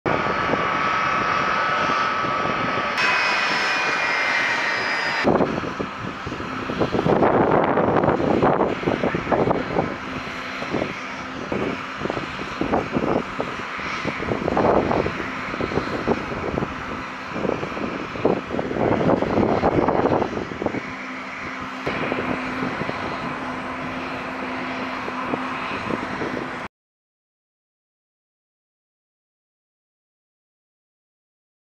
US Secretary of State Blinken makes visit I 토니 블링컨 미국 국무장관 오산공항 도착
Mar. 17
Osan Air Base

Director: Jeon Han
Director of Photography, Editor: Lee Jun Young